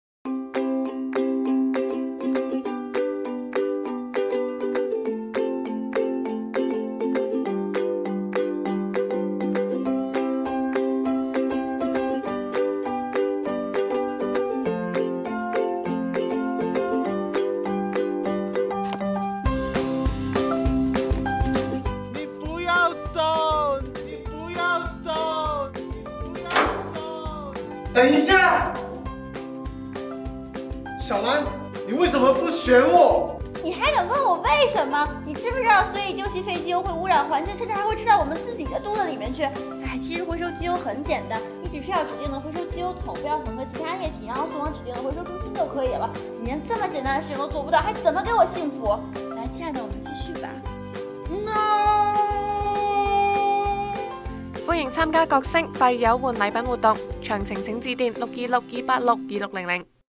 角聲社區中心 / CalRecyle 回收廢機油 (粵) 60s
角聲社區中心 / CalRecyle 回收廢機油
廢機油 換機油盛裝器、
機油漏斗、車間抹布
Used Motor Oil Exchanges for
Oil container, Oil funnel, Shop towel
回收活動 Collection Event
◎ 4/11/2015 (星期六)
10:00am – 3:00pm
O’Reilly Auto Parts
◎ 4/19/2015 (星期日)
2:00pm – 5:00pm
Mission Express Lube
數量有限，先到先得，換完為止。
Quantity is Limited. First Come, First Served Basis!
◎ 須帶至少四夸脫廢機油前來交換
Participants have to bring in at least 4 quarts
of used motor oil